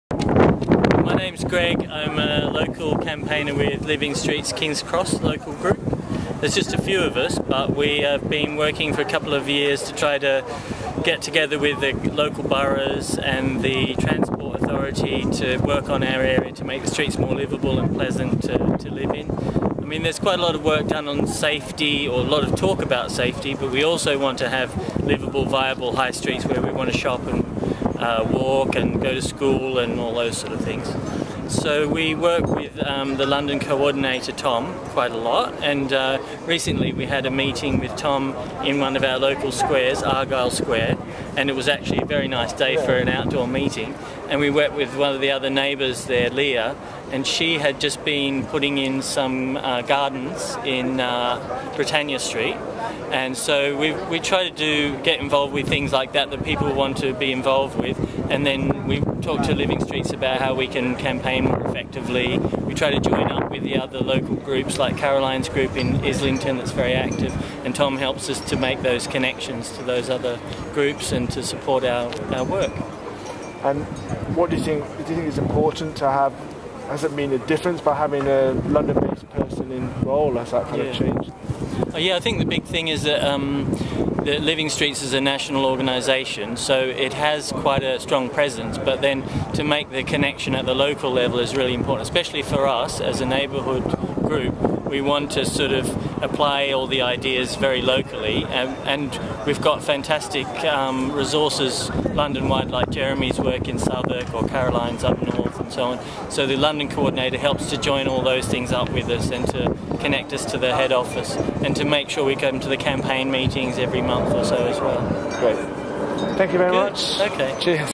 Taking action in King's Cross
Gregory Cowan, chair of Living Streets King's Cross (Camden) Group talks about their campaigning activity this year and how Living Streets has helped